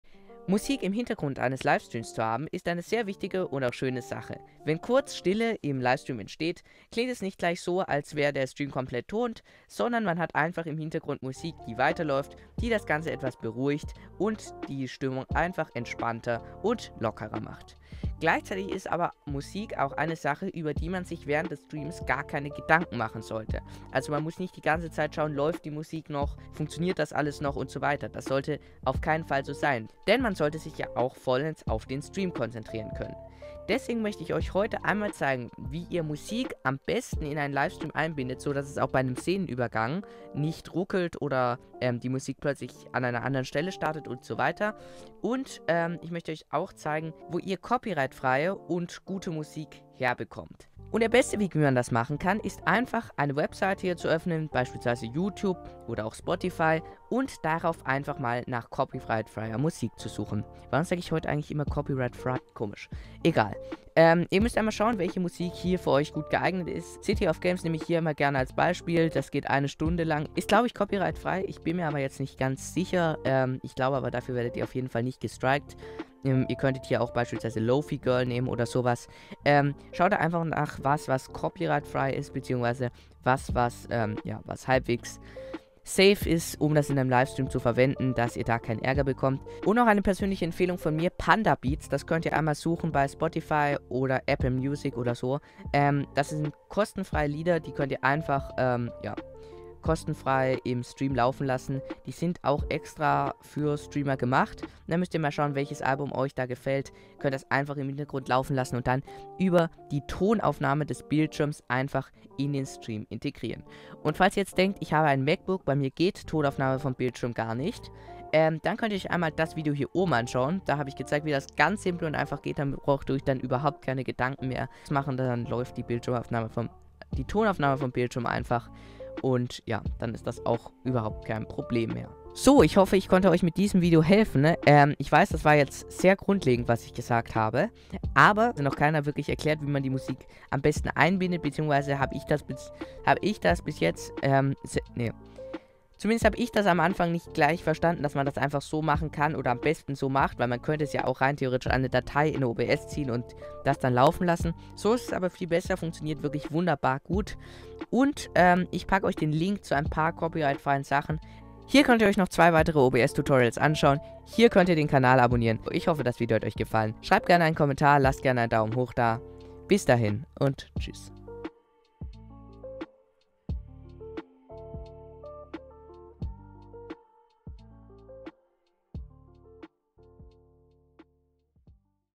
Musik In OBS einbinden Einfach (Problemlösung bei Sehnenübergang)
Ein paar Links zur kostenfreien Musik die die ich euch empfehlen kann Und die ihr beim Streaming problemlos verwenden könnt

Vielen Dank 👍

Ich hoffe ich konnte alle Eure Fragen beantworten und freue mich sehr über Feedback in den Kommentaren 📫